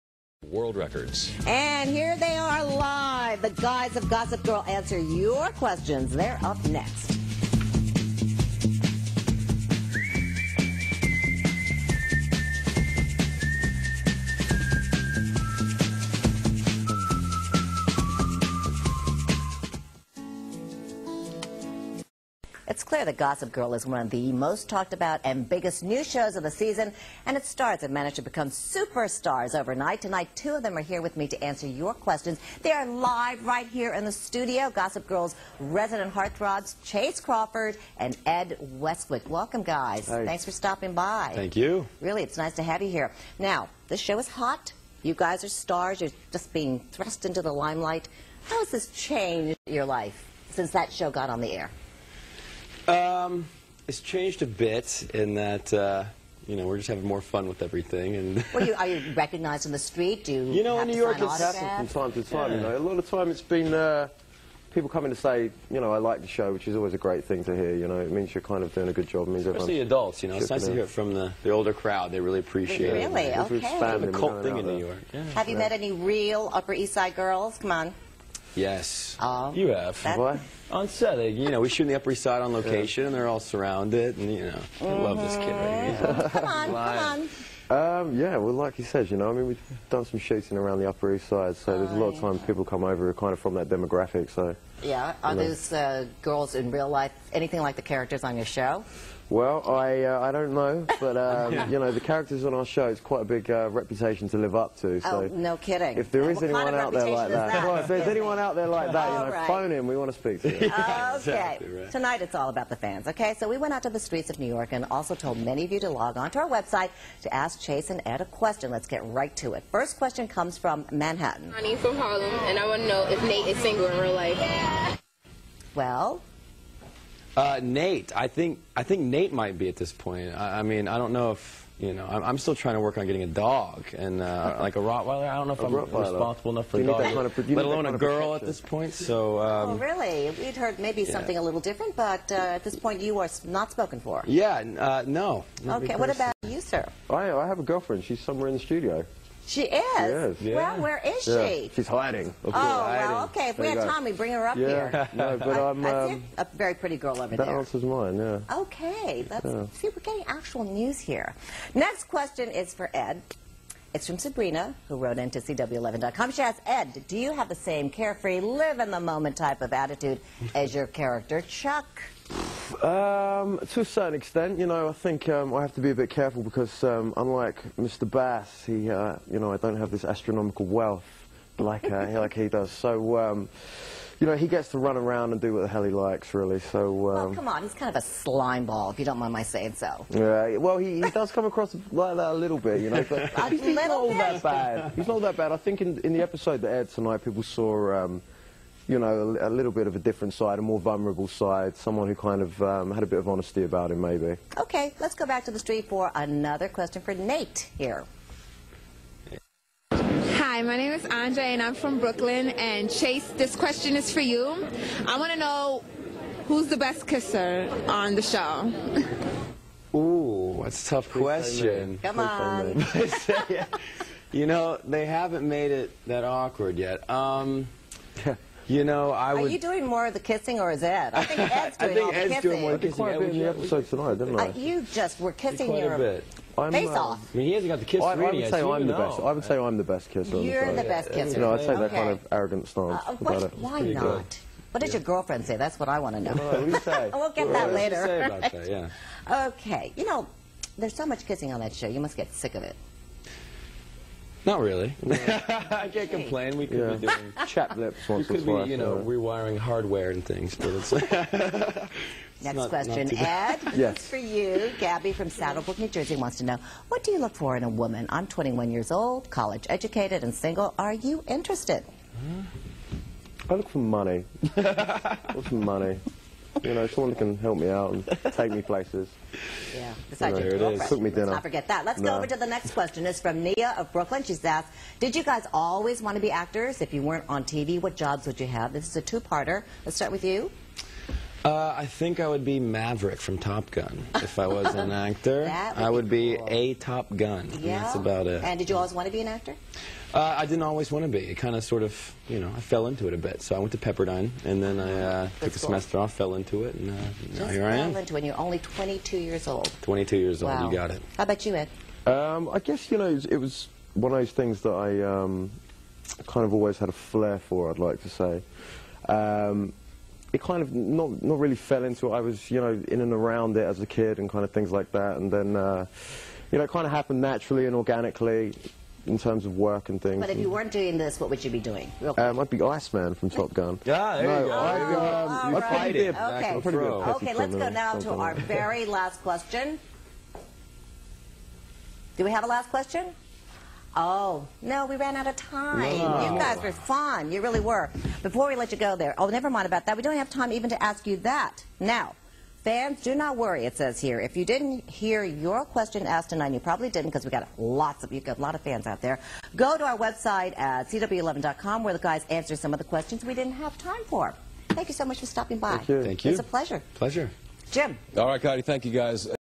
Ed Westwick and Chace Crawford Interview "Who is the best kisser?"
Chace Crawford and Ed Westwick speak about Gossip Girl and other subjects.